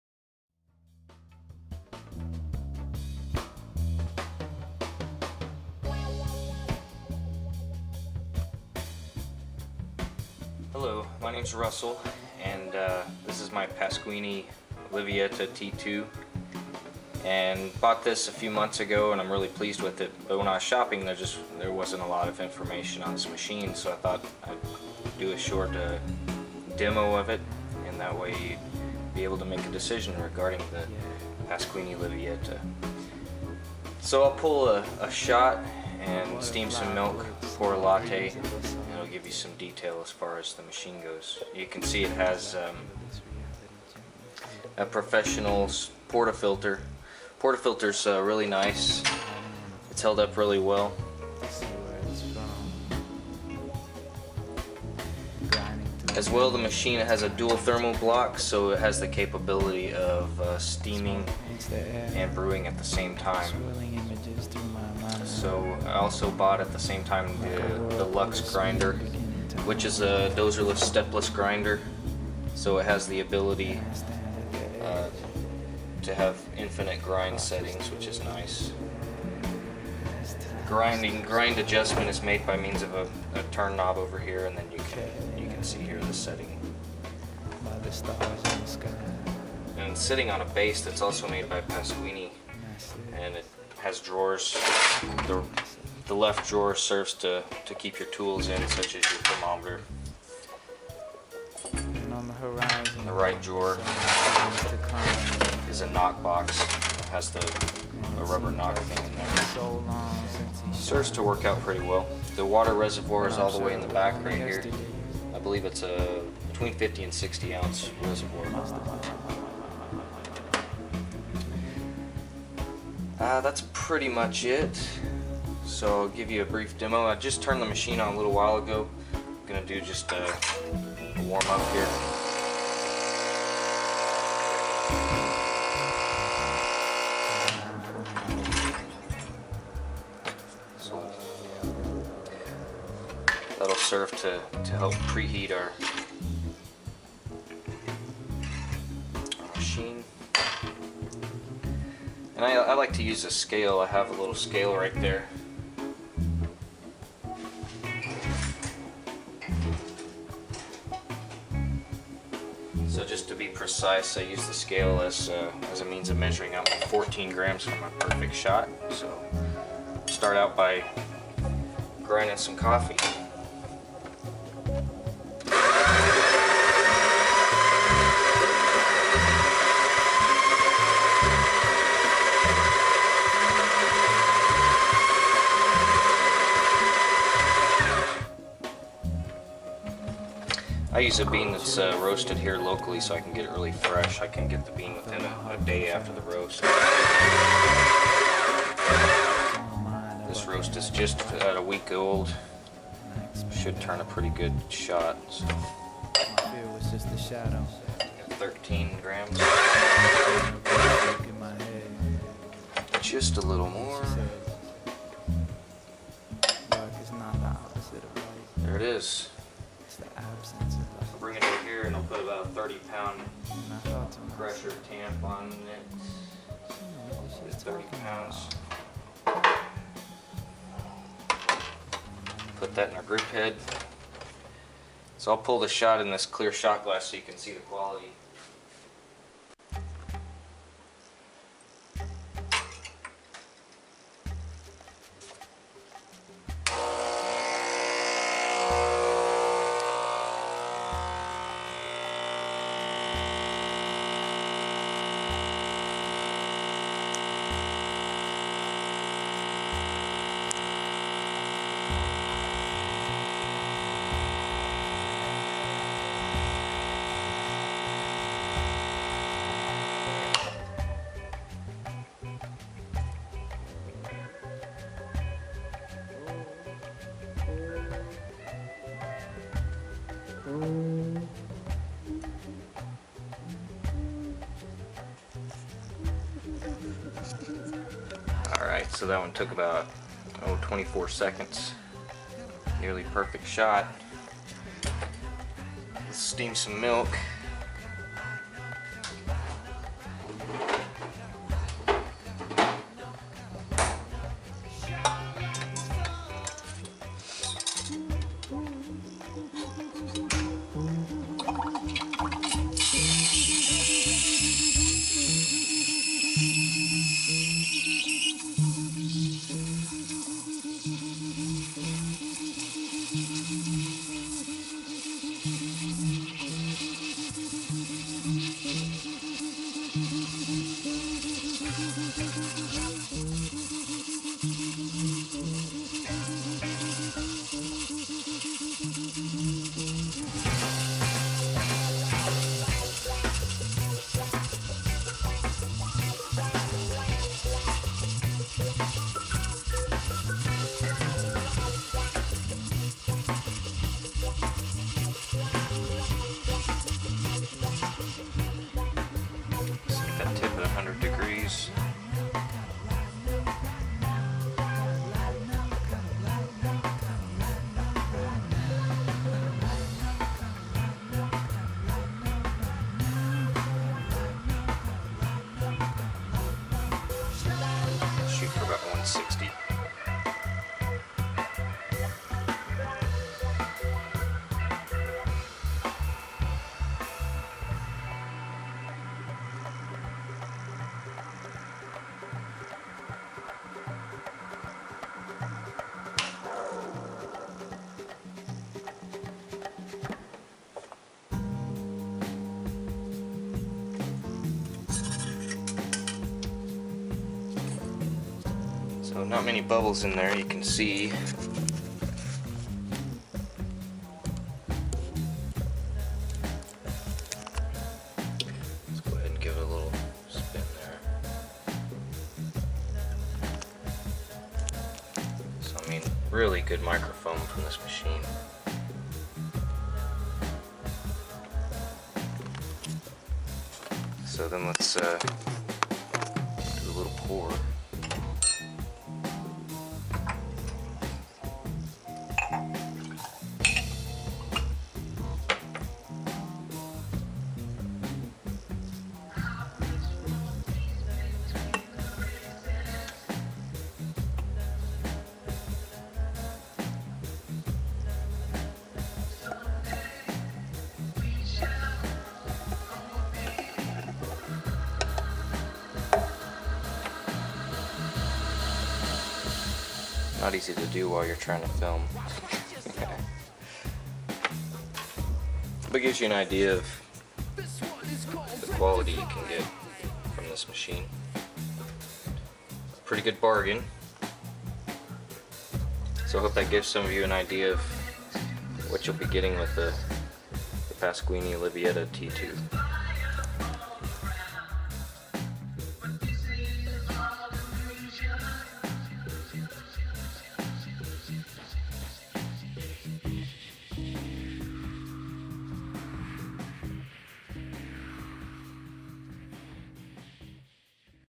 Espresso, Latte with a Pasquini Livietta Espresso Machine
I cover the basics of the Paquini Livietta T2 semi-automatic espresso machine and the Lux burr grinder, then pull a shot and pour a latte.